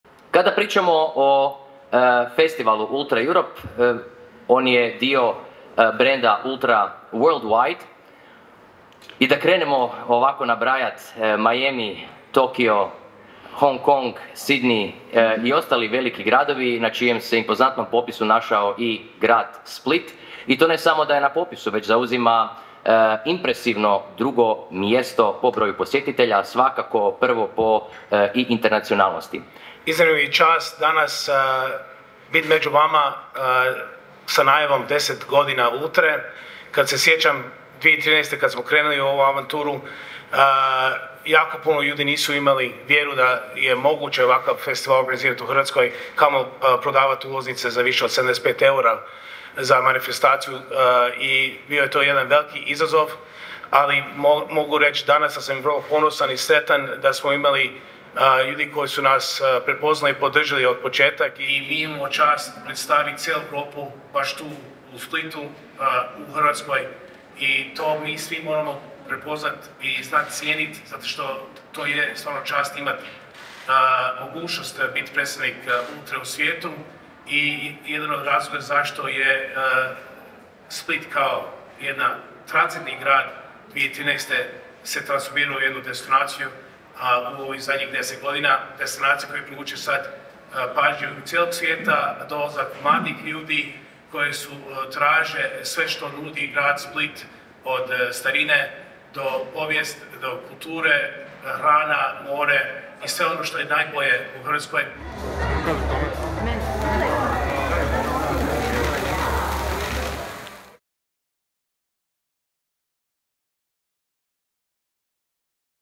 Konferencija za medije povodom 10. Ultra Europe festivala
Split,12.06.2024 - Konferencija za medije povodom 10. Ultra Europe festivala. Na videu: Alek El-Kazal, Joe Bašić. Video: Božidar Vukičević / CROPIX